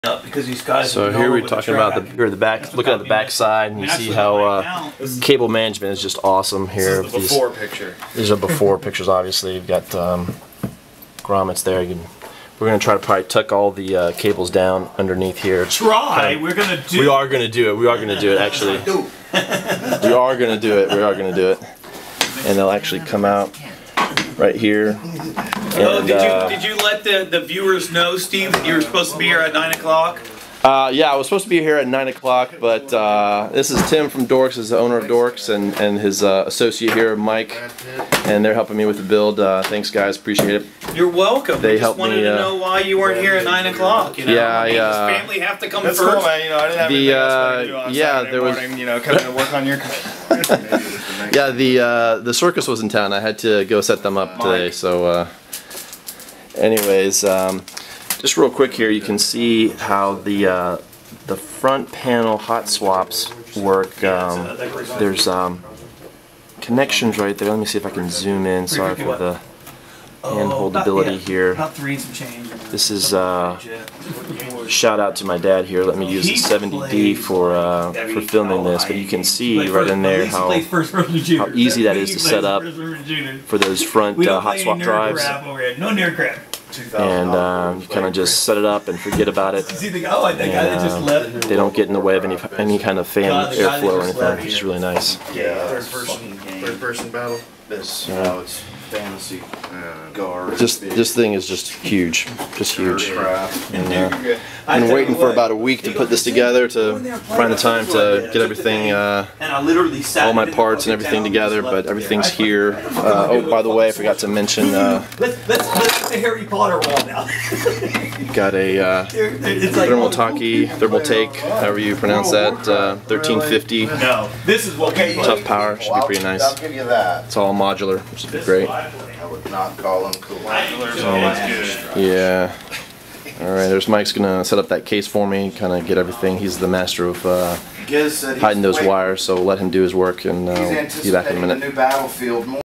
Monster HAF X Build/ It's 9 o'clock! Where's Steve?
Steve was to start the Monster HAF X computer build at Dorks Computers at 9am, but didn't show up until 10am. These aren't banking hours, buddy!